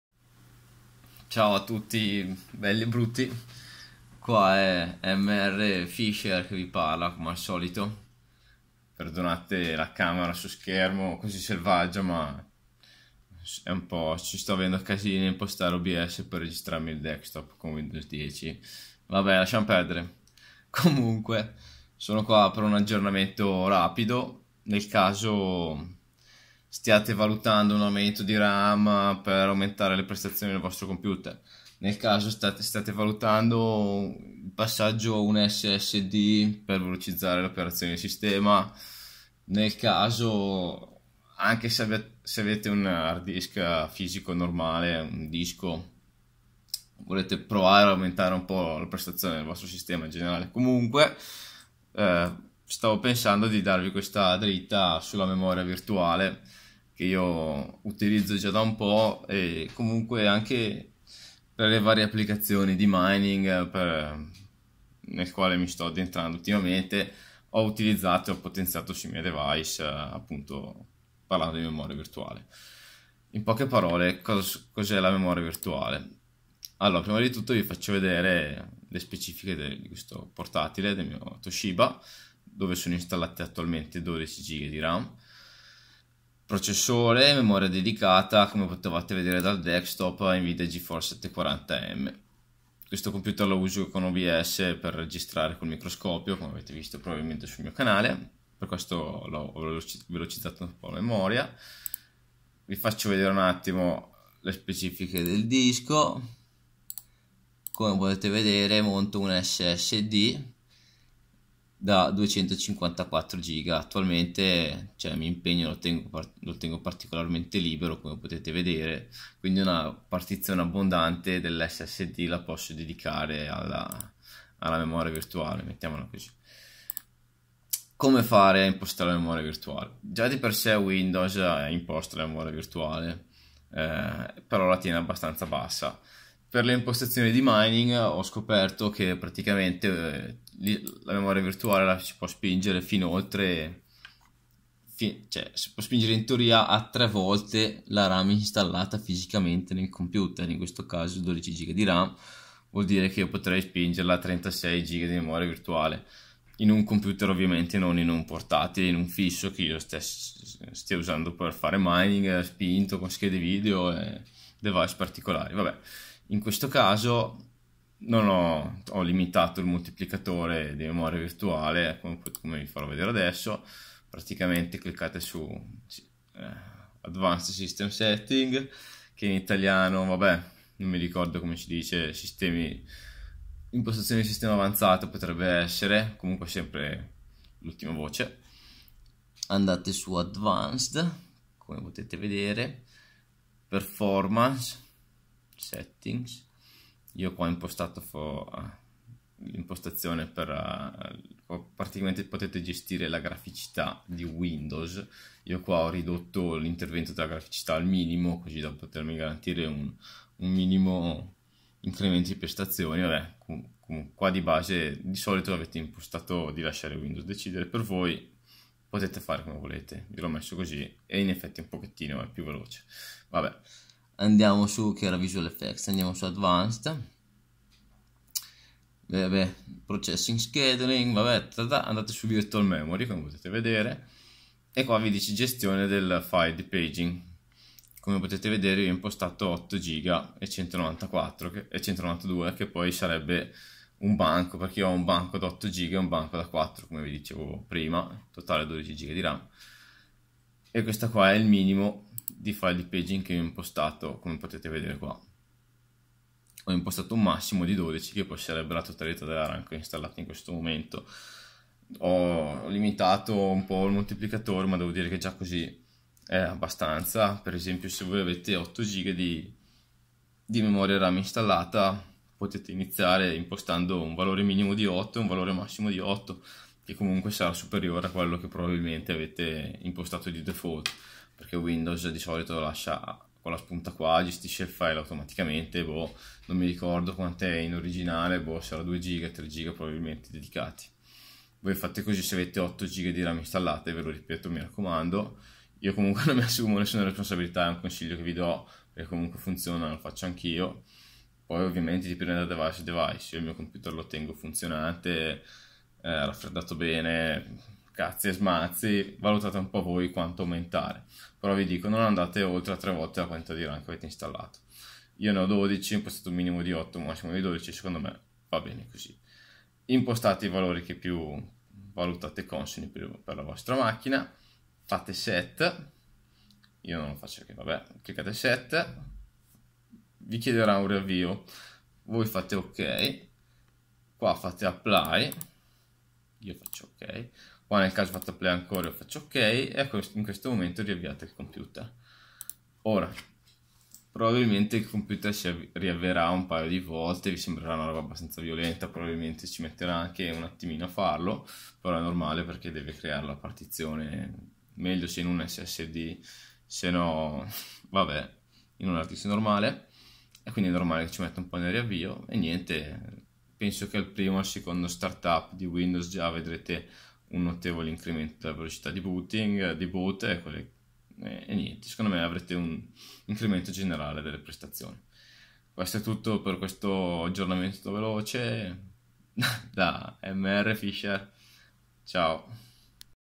Incremento Memoria Virtuale, Paging File, Partizione SSD su Toshiba VG10S Satellite L50A1CU
IMPORTANTE: In generale è sempre consigliato tenere il 25% del disco di sistema libero, aumentare la memoria virtuale occupa parte del disco, impostando la memoria virtuale manualmente non scendere sotto il valore preimpostato di windows e nn salire oltre la vostra RAM installata moltiplicata per 3 volte, anche una piccola modifica impostando il proprio valore di ram è già efficace. Scusate il cam su schermo ma registrare il desktop con obs e win 10 è a oggi un po una chiavica.